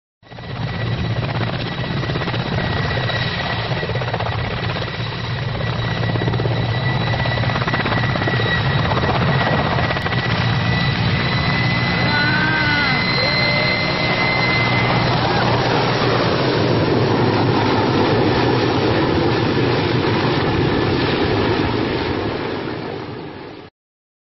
Heli Pilot Rides Nose Wheelie.flv
Russian Helicopter Pilot Rides Nose Wheelie